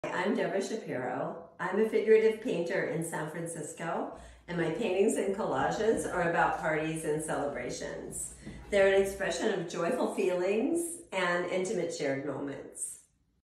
Artist Debra Shapiro at Superfine Art Fair San Francisco
“My paintings and collages are about parties and celebrations.” - Debra Shapiro
Look out for her work at The Superfine Art Fair from November 16th-19th at San Francisco booth 50